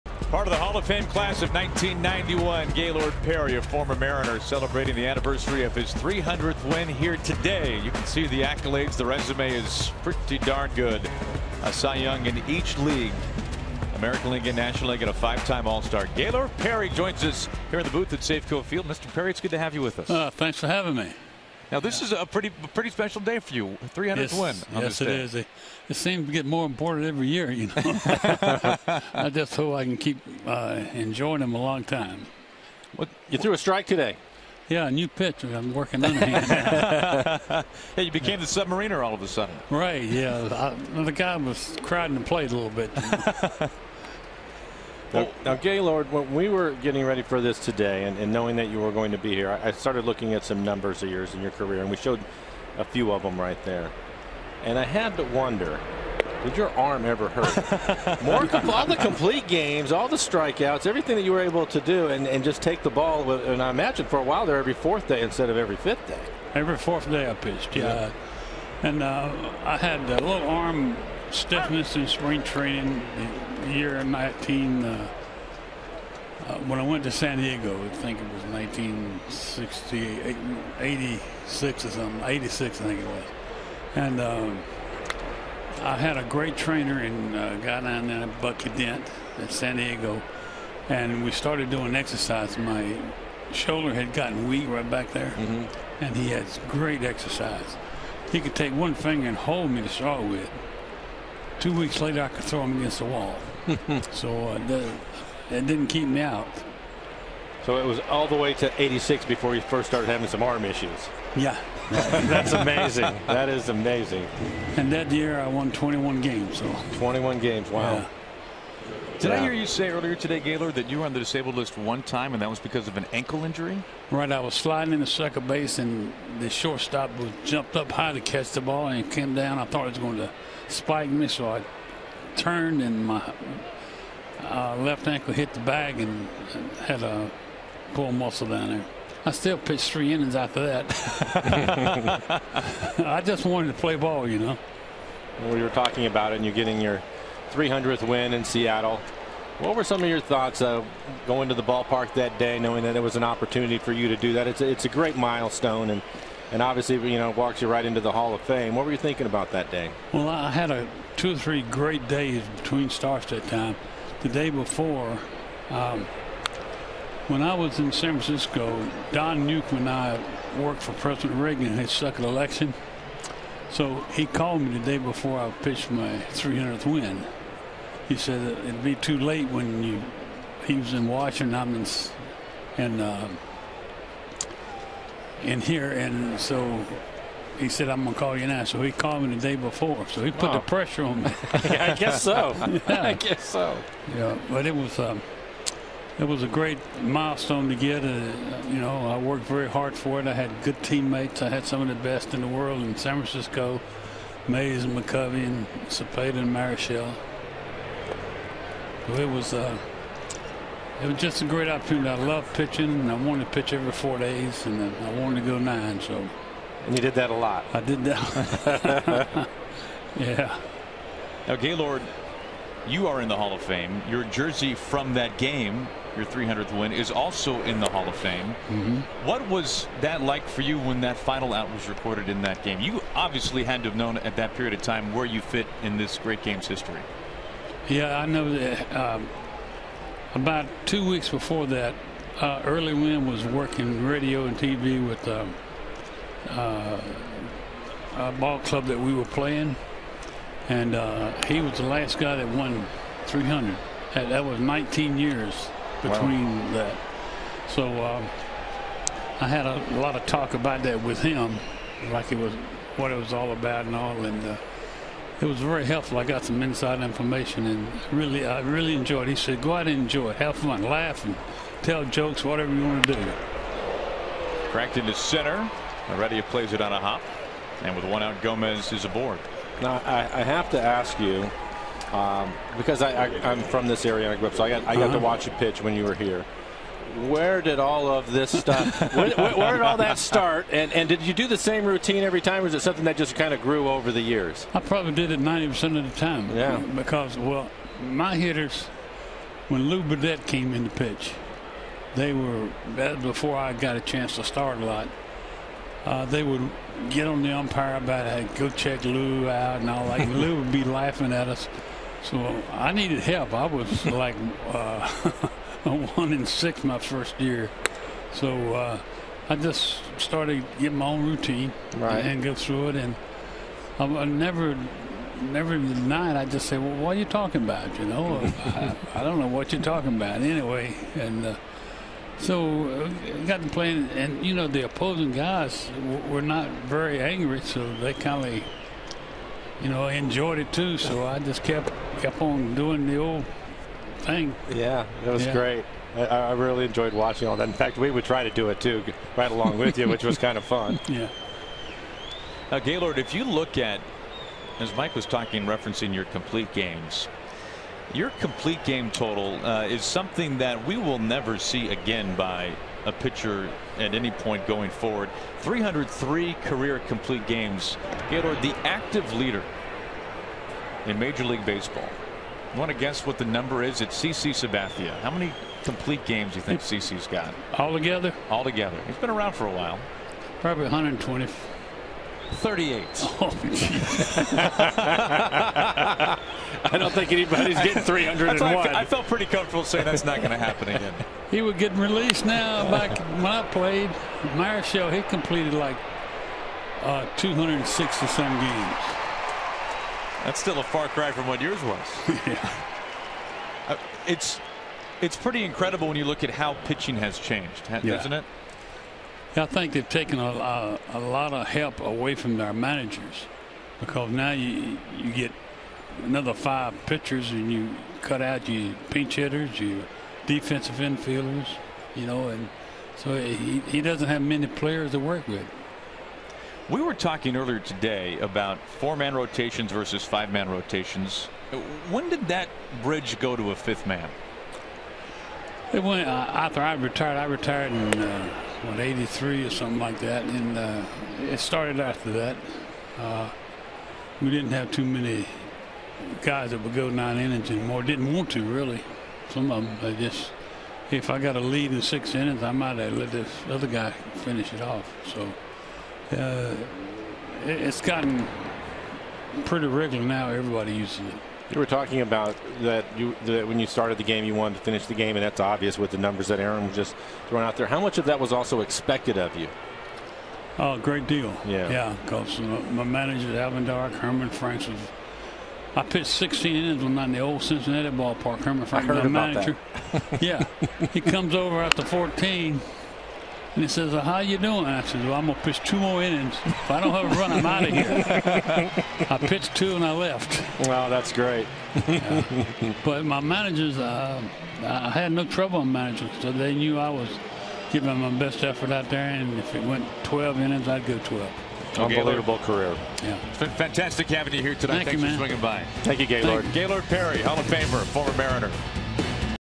TEX@SEA: Gaylord Perry discusses his historic career
Gaylord Perry joins the booth to discuss his Hall of Fame career and his time playing with the Seattle Mariners